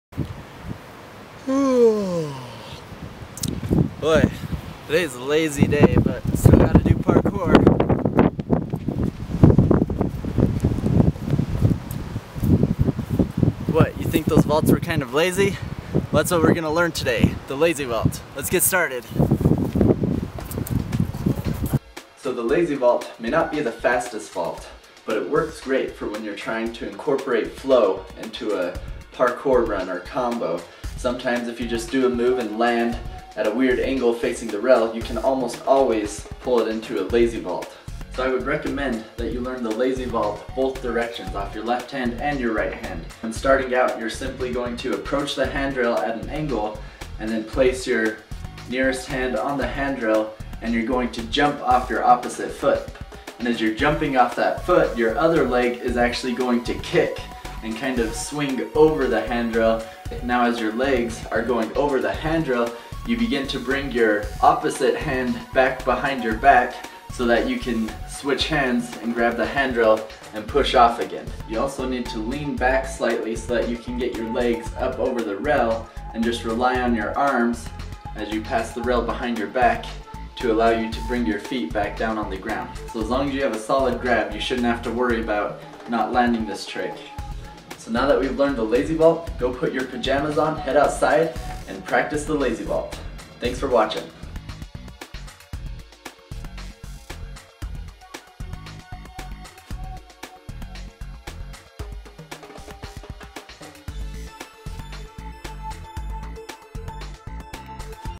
How to LAZY VAULT - Parkour Tutorial
Having a lazy day? Learn the lazy vault a simple yet effective parkour move.

Best Parkour Shoes, Apparel, Fitness Equipment and Film Gear.